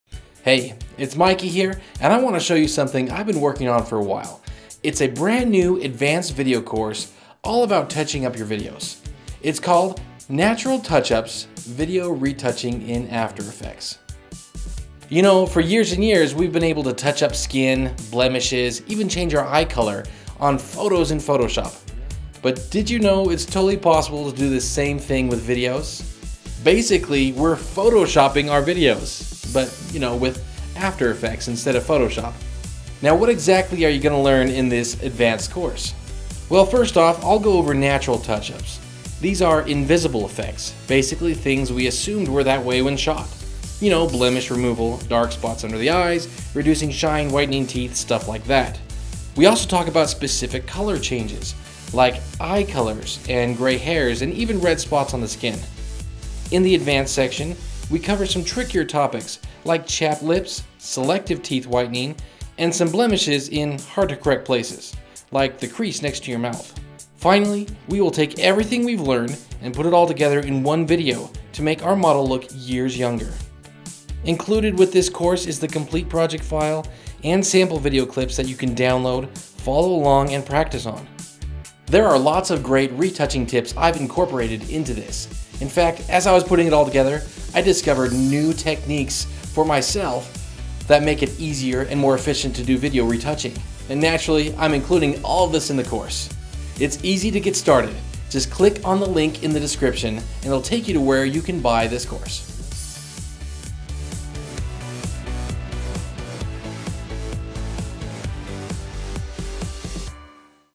Natural Touchups Advanced Video Course
New advanced video training in After Effects, available NOW!!!

You know for years and years we’ve been able to touchup skin, blemishes or even eye color on our photos in Photoshop?  Well, did you know its totally possible to do this same thing on Video.  Basically we are “photoshoping” our videos, but you know, with after effects instead of photoshop.  There are lots of great retouching tips incorporated into this, In fact as I was putting it together, I discovered new techniques to make retouching easier and more efficient, And naturally, i’ve shared them all with you in this course. For awesome tools and plugins for After Effects, go